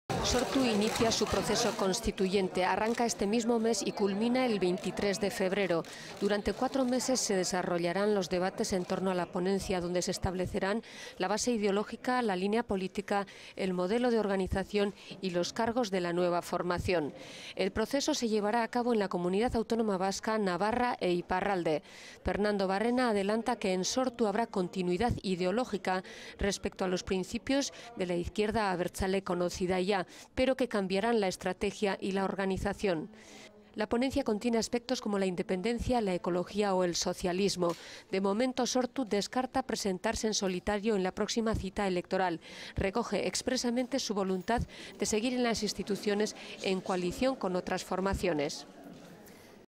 Sortu celebrará su Congreso constituyente el 23 de febrero
Sortu detalla sus bases organizativas e ideológicas en una ponencia. Sortu elegirá a su representantes el 9 de febrero y celebrará su Congreso constituyente el 23 de ese mes.